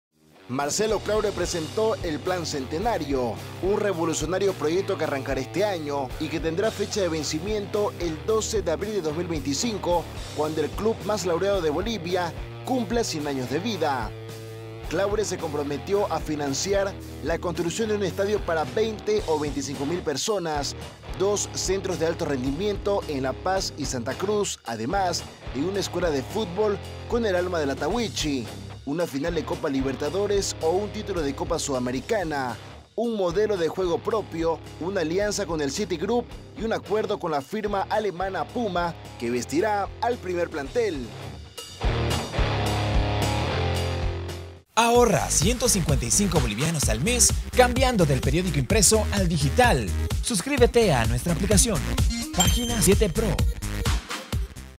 ¡Plan histórico! Claure ofrece estadio, CAR... y título internacional en 2025
¡Plan histórico! @marceloclaure ofrece estadio, CAR... y título internacional en 2025.